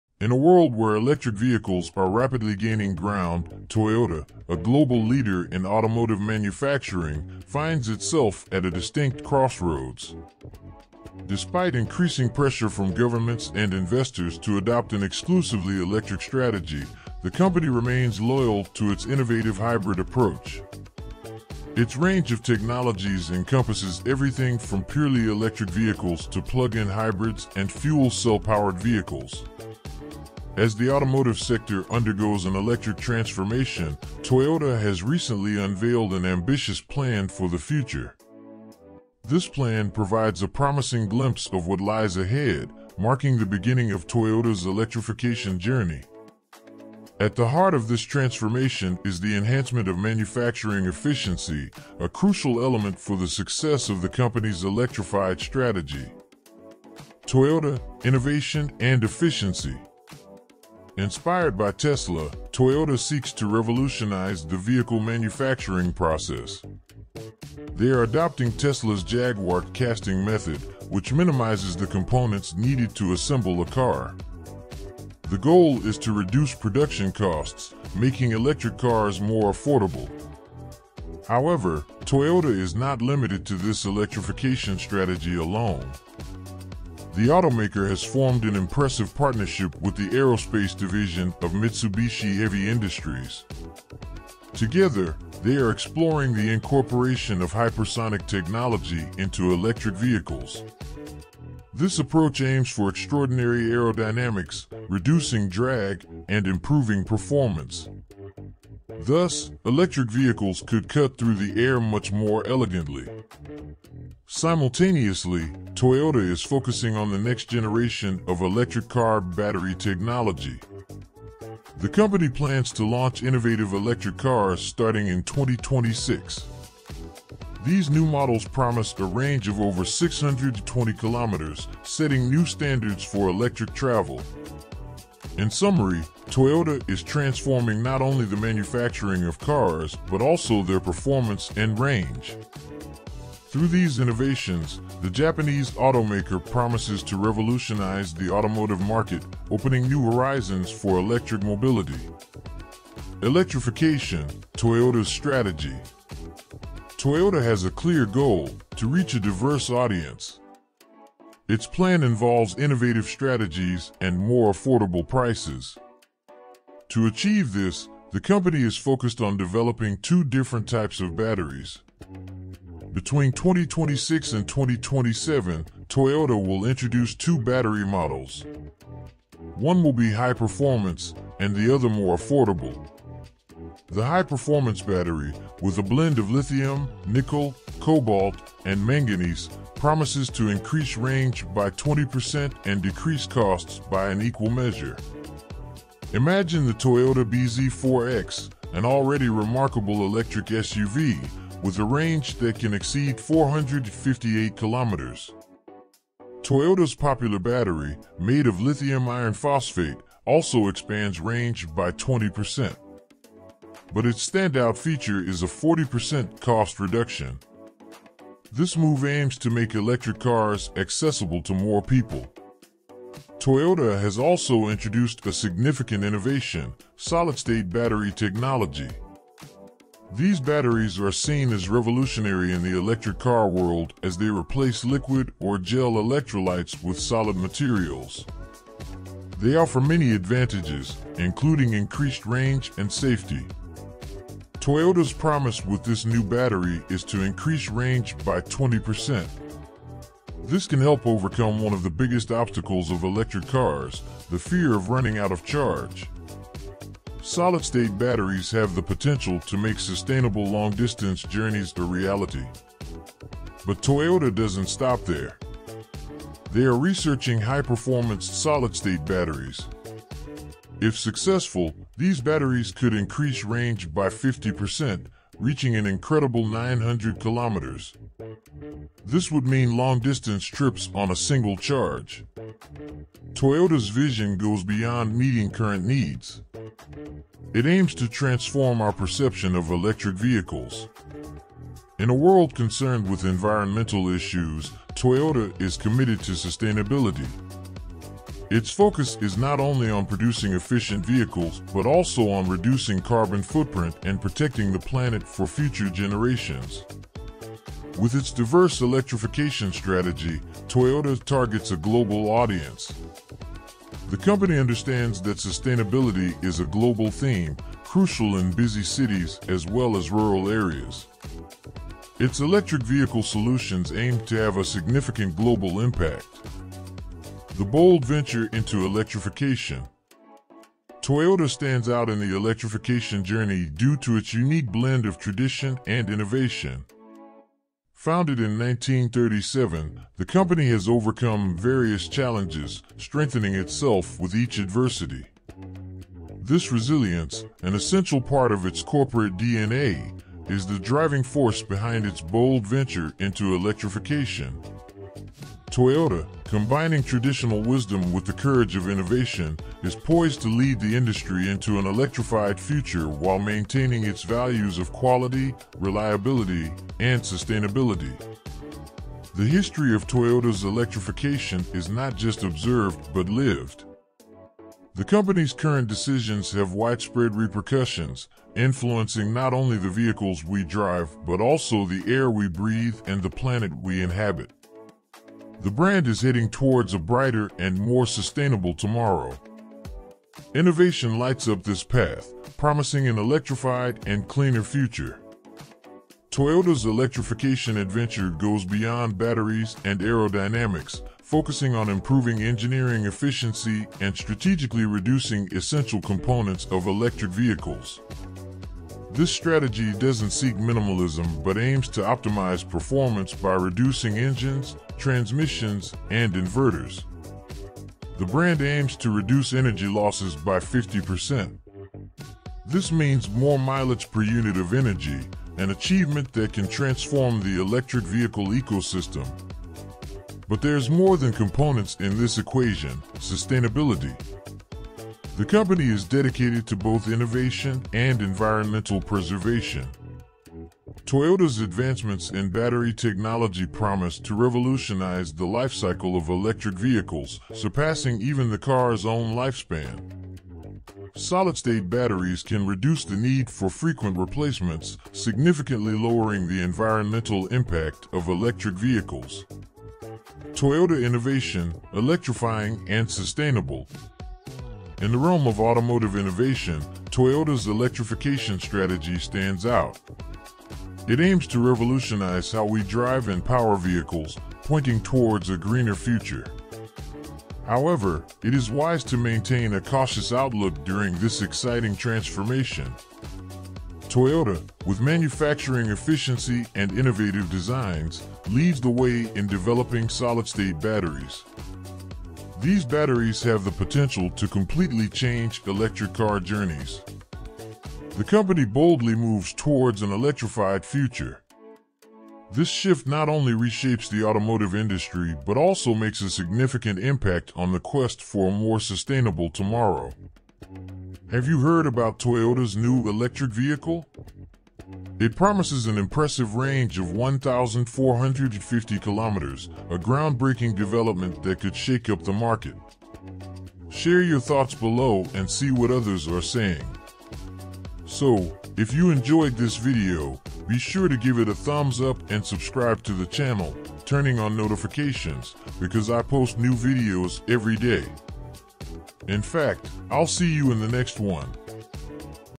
TOYOTA'S ELECTRIFIED ERA: UNVEILING THE GREEN REVOLUTION!
Dive into the future as Toyota sparks a revolution in the automotive landscape! Join us in uncovering the secrets behind Toyota's electrified era, promising a green revolution that reshapes how we drive. Buckle up for innovation!